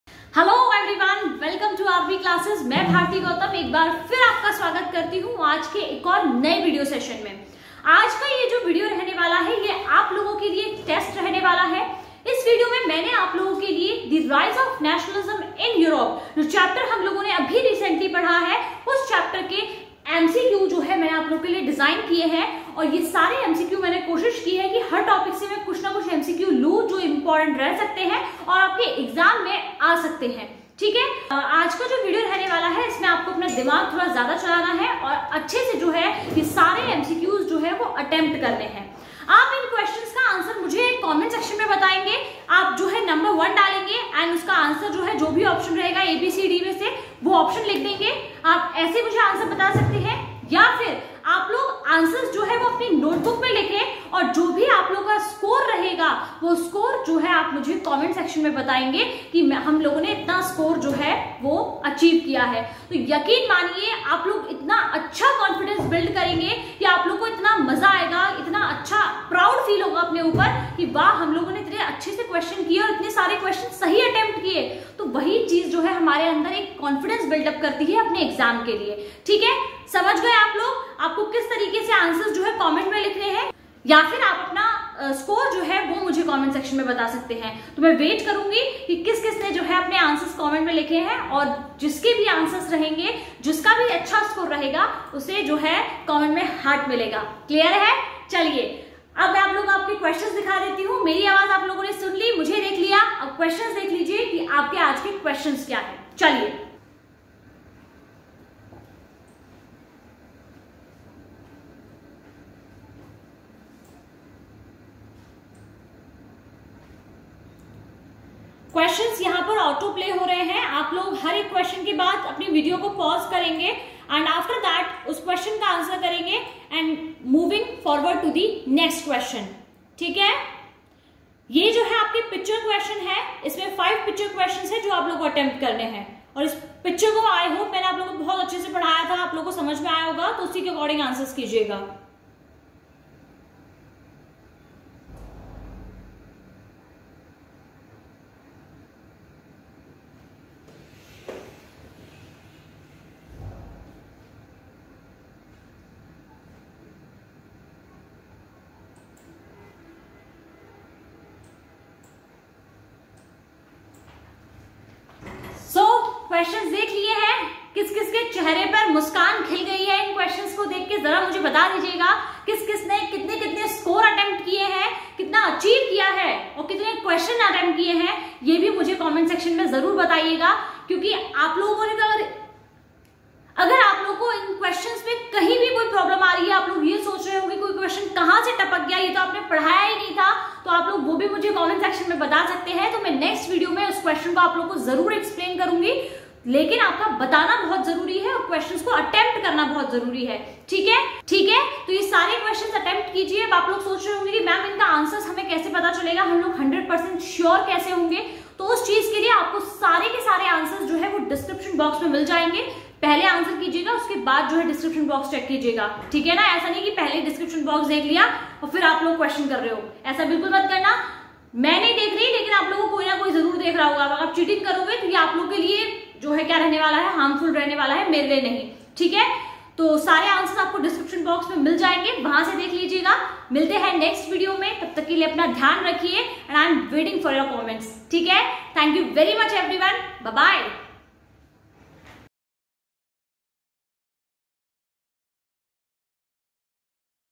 Class 10 History Important Questions| The Rise of Nationalism in Europe MCQ| R B Classes
Answers:
Q1. c , Q2. b, Q3. a, Q4. d, Q5. b, Q6. c, Q7.Picture Qs 1. c, 2.c, 3.d, 4.c, 5.c
Q8. a, Q9. d, Q10. b, Q11.d, Q12. d, Q13. c, Q14. b, Q15. b, Q16. b, Q17. B